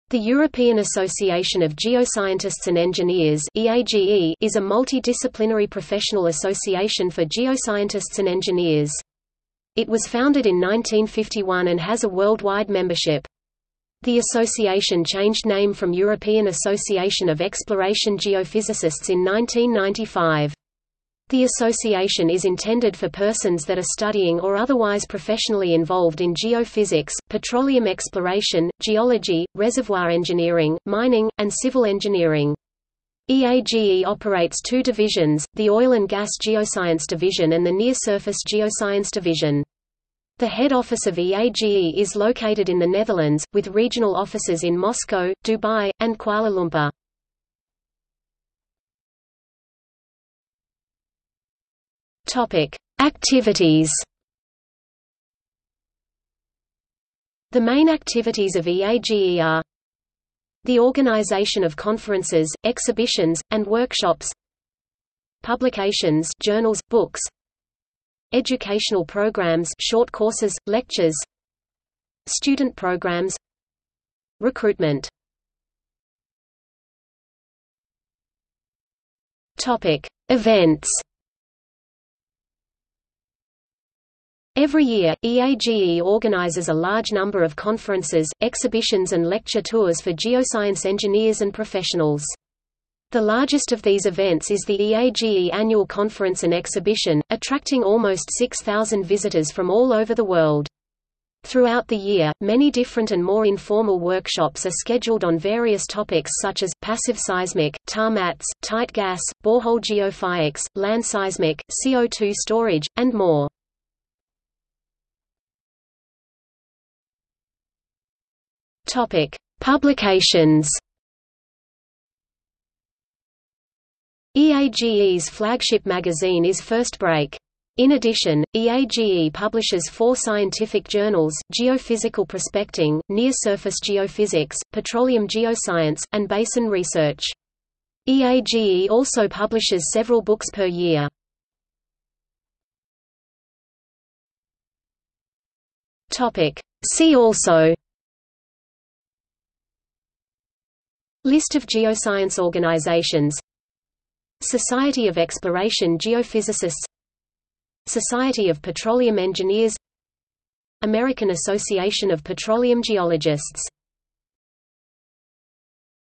European Association of Geoscientists and Engineers | Wikipedia audio article
00:00:48 1 Activities
00:01:16 2 Events
00:01:55 3 Publications
00:02:22 4 See also

- Socrates

SUMMARY
The European Association of Geoscientists and Engineers (EAGE) is a multi-disciplinary professional association for geoscientists and engineers. It was founded in 1951 and has a worldwide membership. The association changed name from European Association of Exploration Geophysicists in 1995. The association is intended for persons that are studying or otherwise professionally involved in geophysics, petroleum exploration, geology, reservoir engineering, mining, and civil engineering. EAGE operates two divisions: the Oil & Gas Geoscience Division and the Near Surface Geoscience Division. The head office of EAGE is located in the Netherlands, with regional offices in Moscow, Dubai, and Kuala Lumpur.